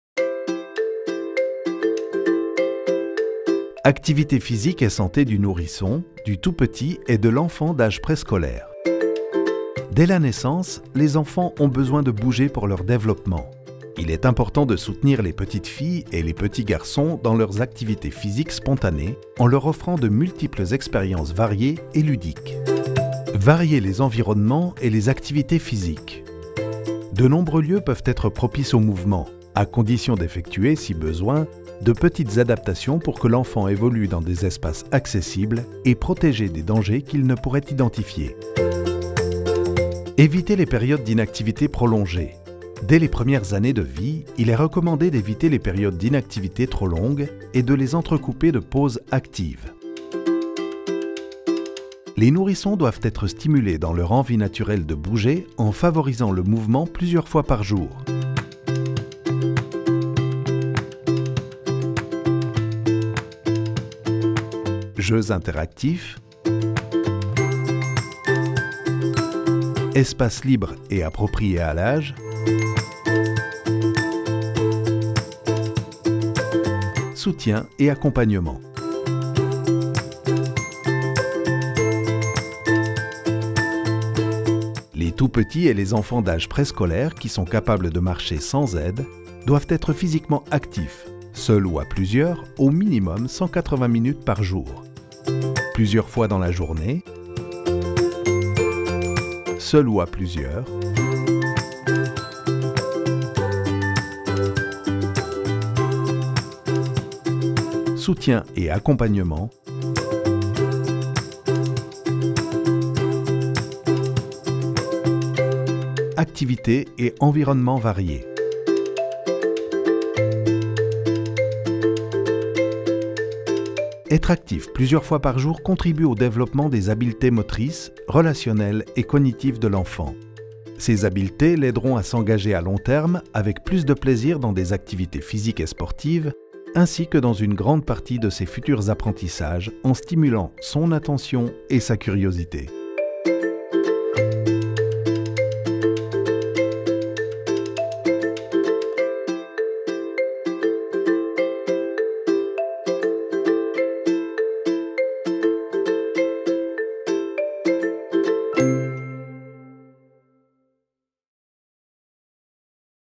Recommandations d’activité physique pour les nourrissons et les enfants d‘âge préscolaire
Dès la naissance, tous les enfants ont besoin de bouger pour leur développement. Être actif plusieurs fois par jour a des effets positifs sur la santé physique, le bien-être psychique et social.